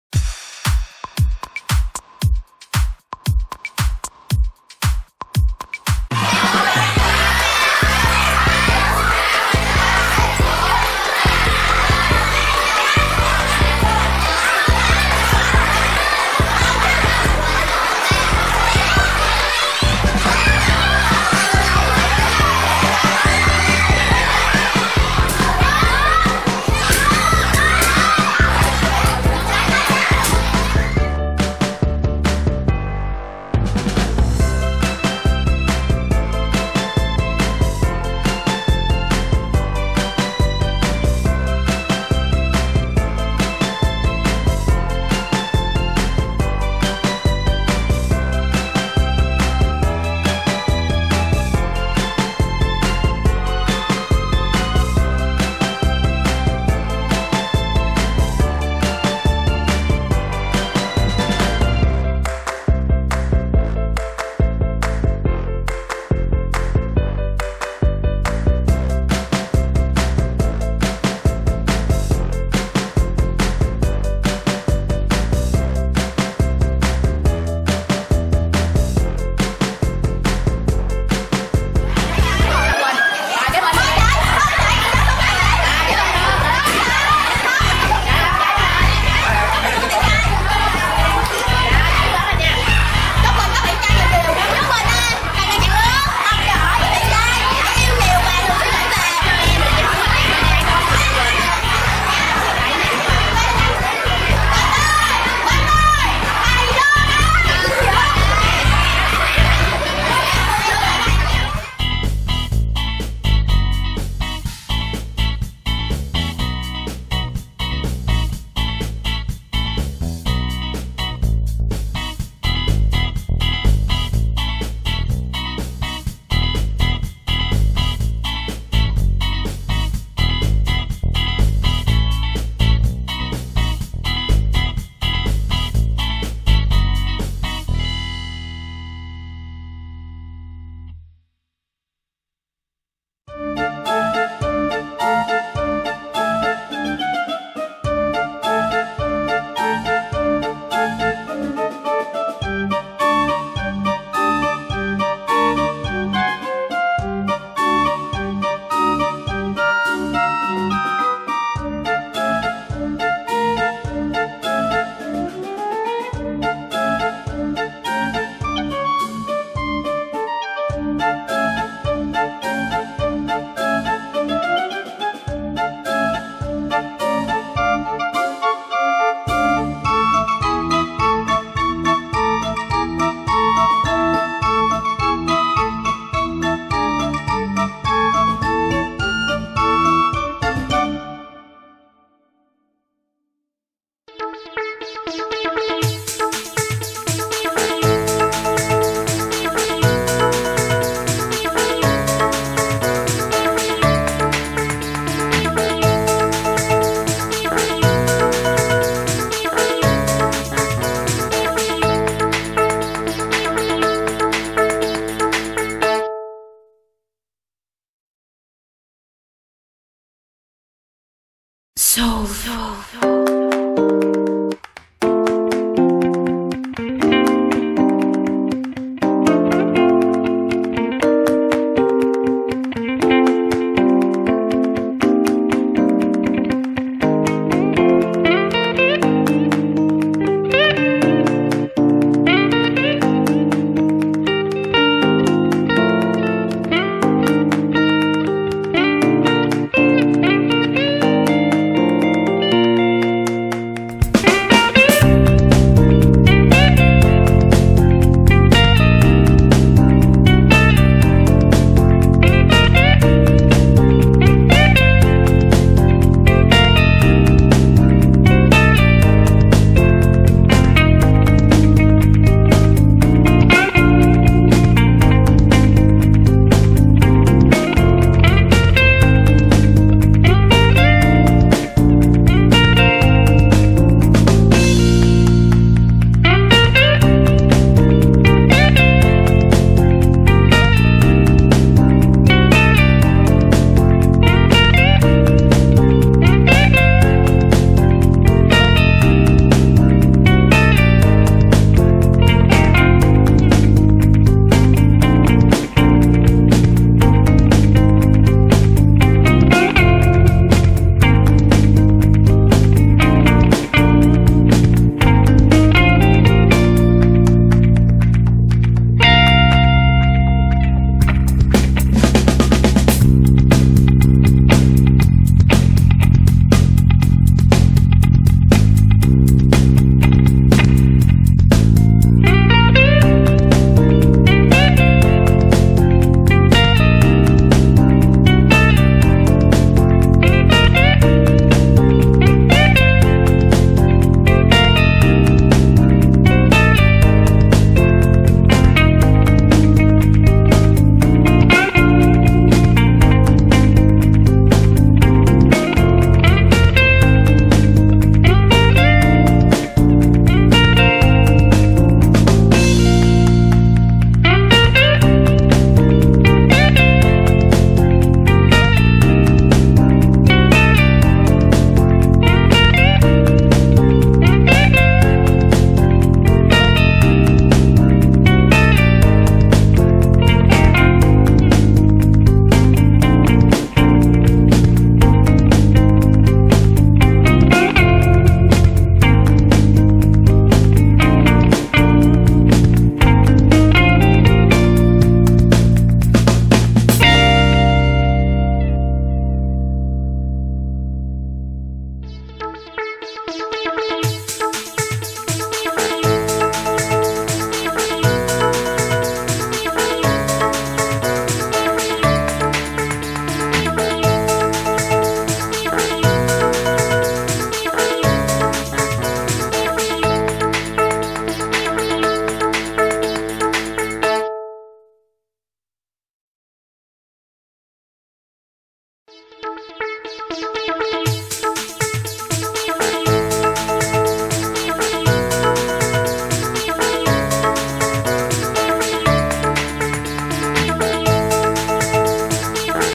Tiệc sinh nhật của chị hàng xóm thật vui | Bao Minh Channel
Hi, chào các anh chị & các bạn,
Chủ nhật vừa rồi mình và em Linh đi sinh nhật chị Quỳnh nè, vui lắm. Sau đó, chơi trò giành ghế nữa đó, hihi.
Mình có quay thêm 1 lớp các anh chị khác cũng chơi trò này.
Cảm ơn các anh chị & các bạn rất nhiều nhé.